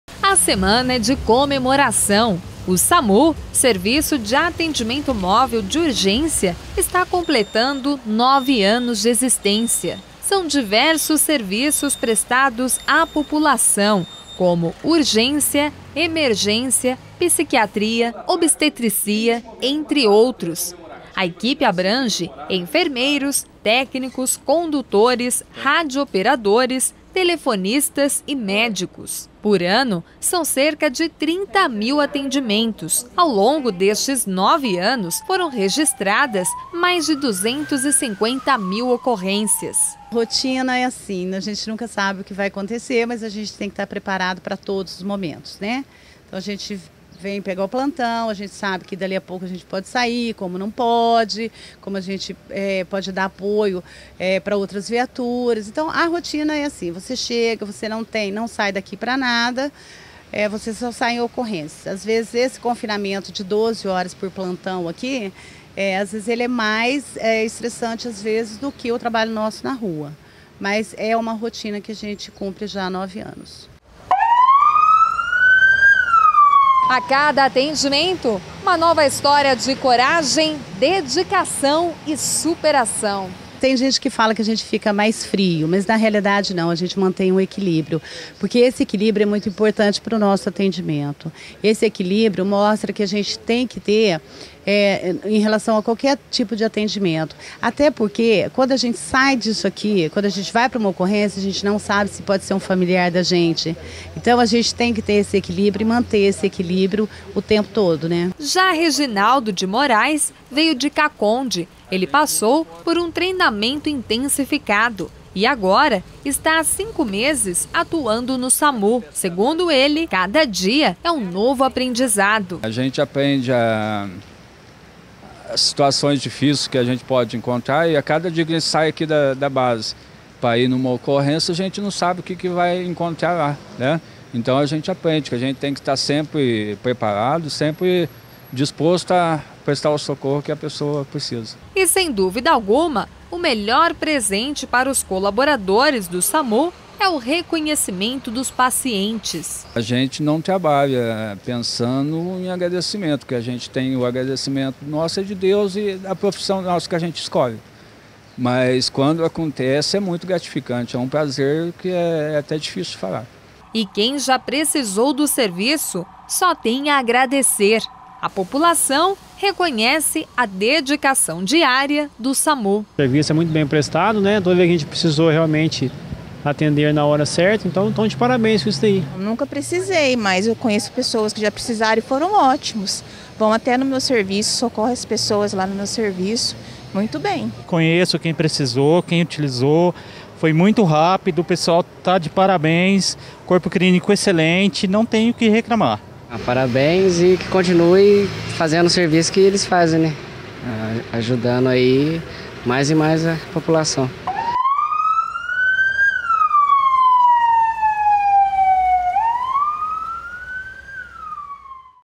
ANIVERSÁRIO SAMU
SAMU completa nove anos em Poços de Caldas.